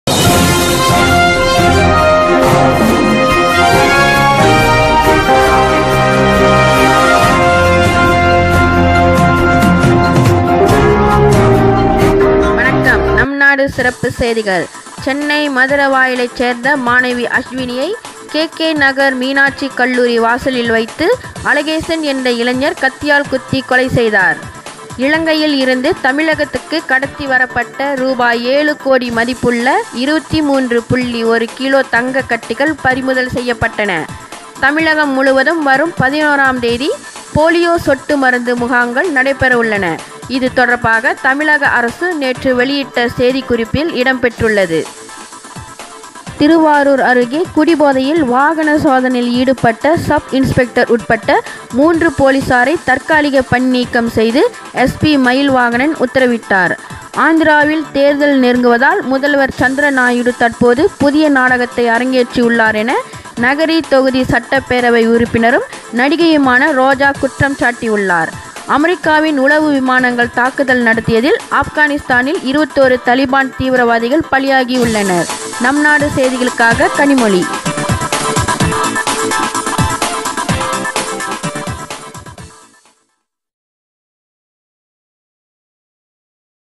நம்நாடு உலக செய்திகள் (9/3/2018)
நம்நாடு உலக செய்திகள் கல்லூரி மாணவி கத்தியால் குத்தி கொலை மற்றும் பல செய்திகளுடன் காணத்தவறாதீராகள்